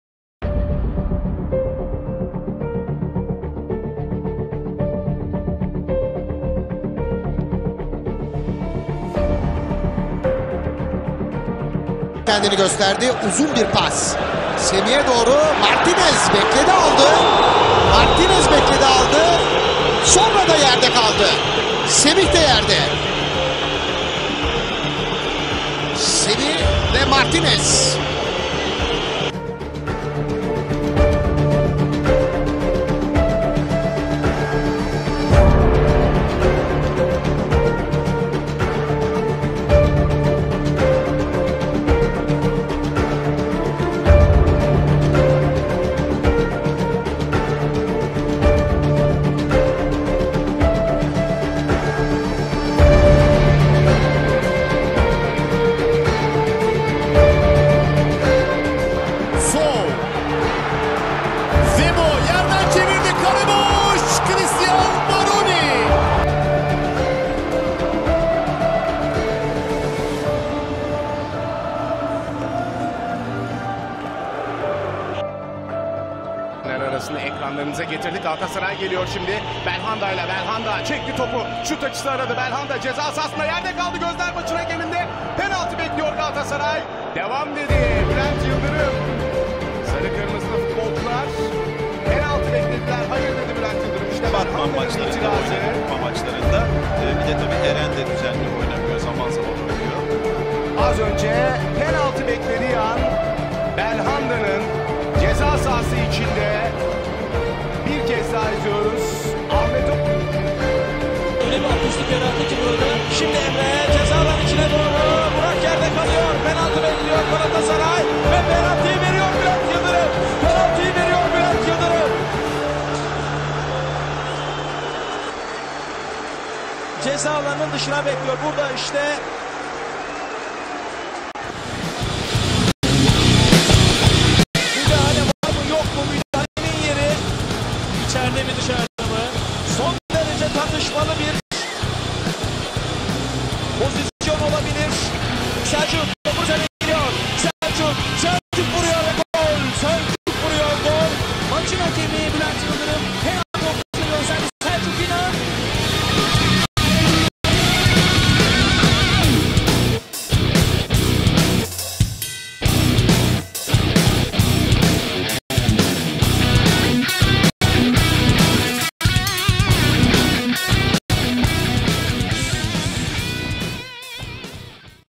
BÜLENT YILDIRIMDAN MAÇA DAMGA VURAN 5 HAKEM HATASI
Bu akşamki Fenerbahçe - Galatasaray maçındaki tartışmaya açık kararlarının ardından Bülent Yıldırım'ın hakem hatalarını sizin için derledim. Böyle bir liste ortaya çıktı. İşte karşınızda Bülent Yıldırım'dan maçlara damga vurmuş 5 hakem hatası.

🌍 Gündemde ne var ne yok önce sen öğren🌍
🔔 Zili açarak bildirim gelmesini sağlayabilirsin🔔
👍 Emeğe saygı bir beğeni ve yorum bırakırsan memnun olurum👍